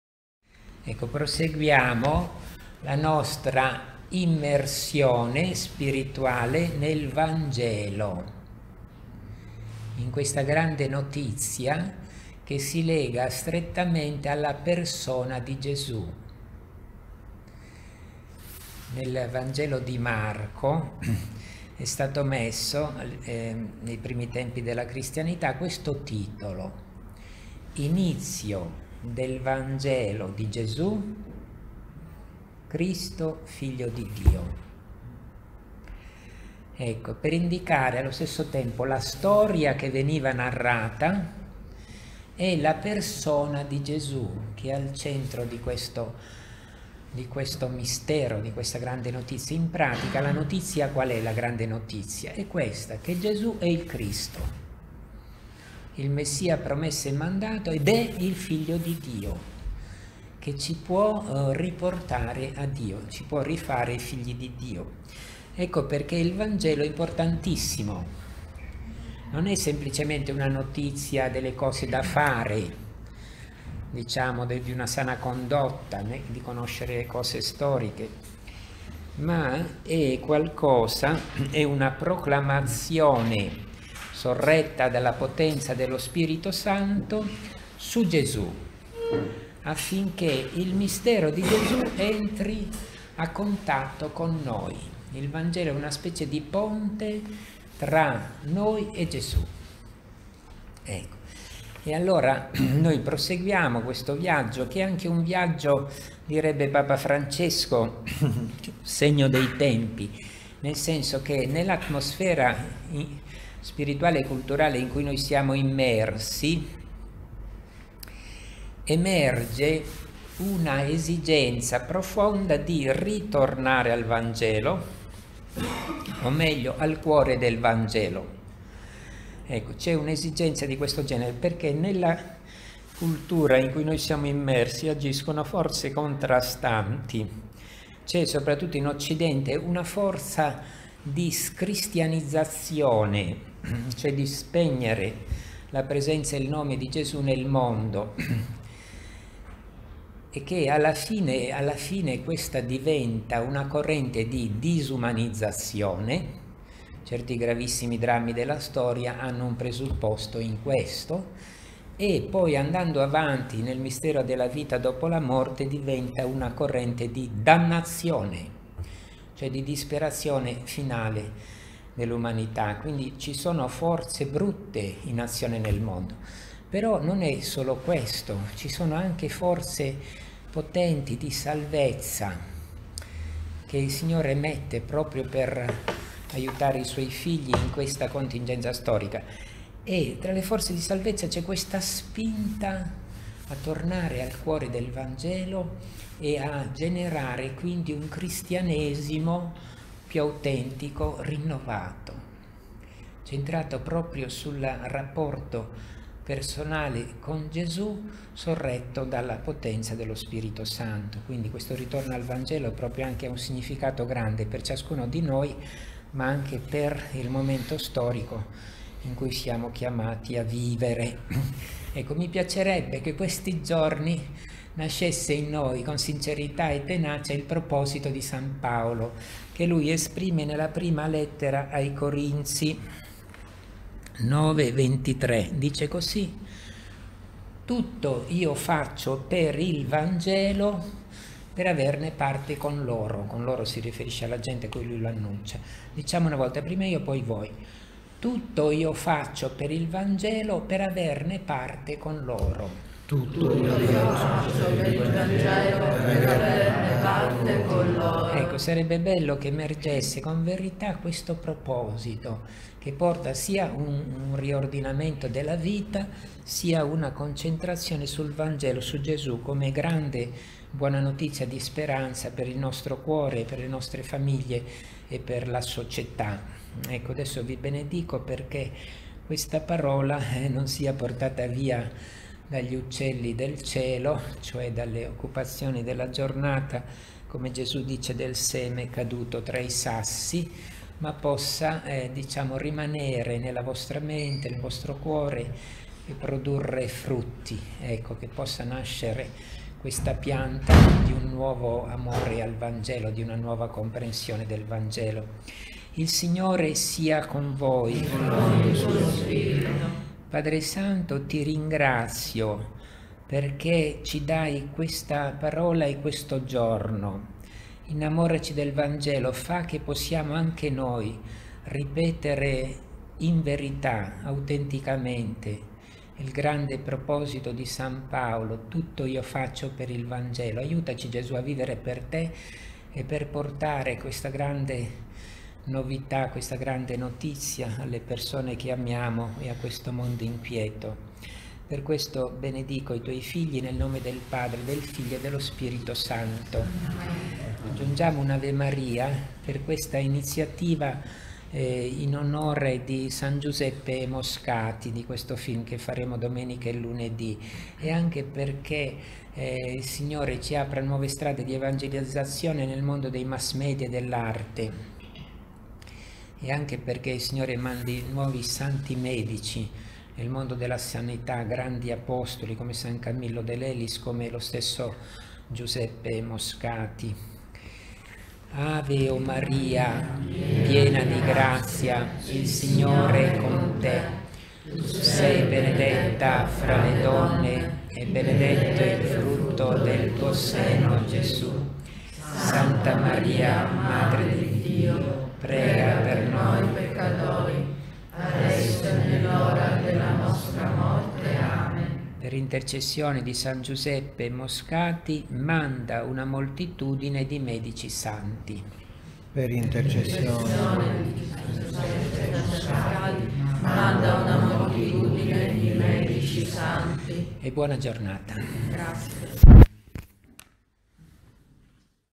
Meditazione 23 Gennaio 2018 - Proclamare il Vangelo - Padre Amedeo Pascucci
Il tempo è compiuto il Regno di Dio si è avvicinato
Pregate dunque il Padrone della messe perchè mandi operai alla sua messe Mt 9, 37-38; Lc 10, 2
Manda Signore apostoli Santi nella tua chiesa
Spirito di Santità facci Santi, presto Santi e grandi Santi
Padre Amedeo Pascucci